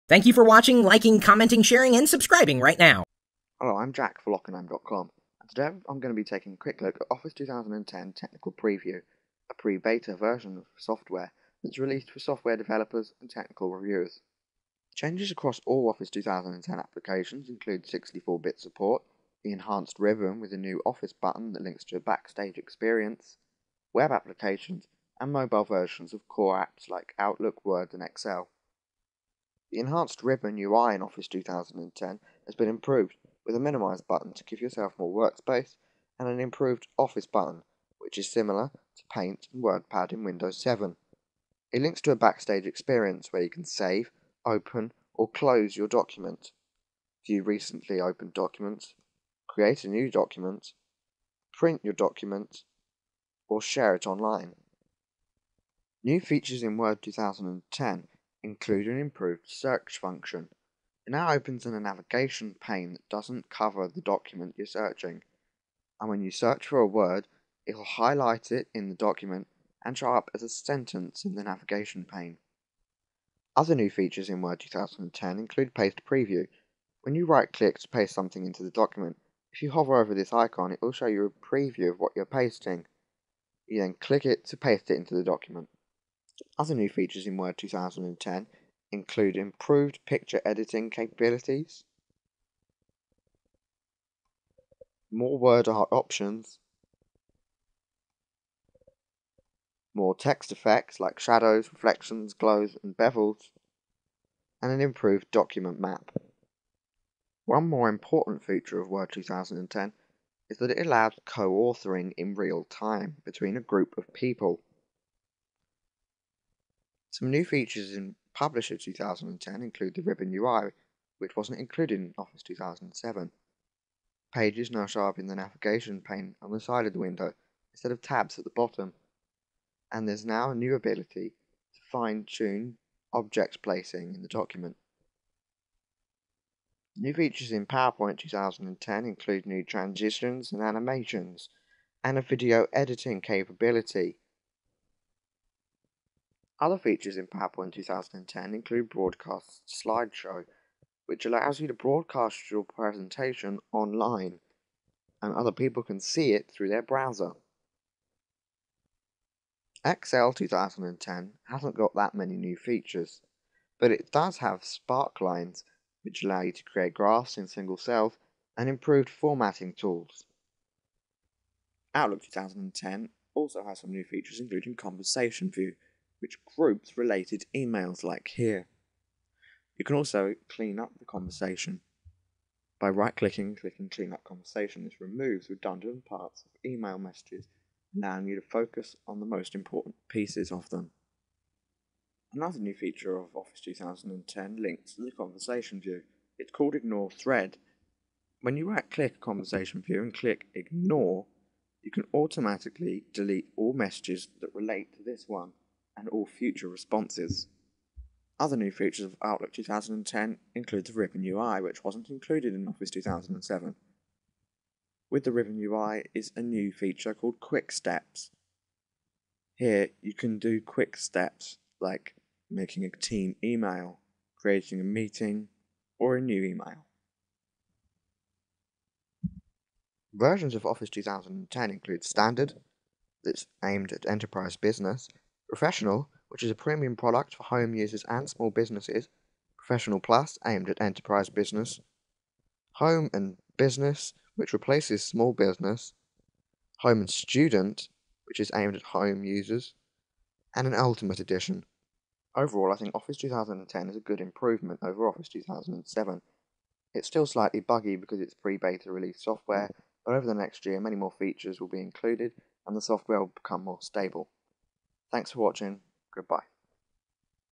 Microsoft Office 2010 Screencast
- Recently I threw down the gauntlet, asking all of you to submit your best screencasts. I plan to highlight the best of the best here in my channel, and help promote yours. Jack has sent a few in to us now, and they're fantastic. This one gives you a quick (but excellent!) overview of Microsoft Office 2010.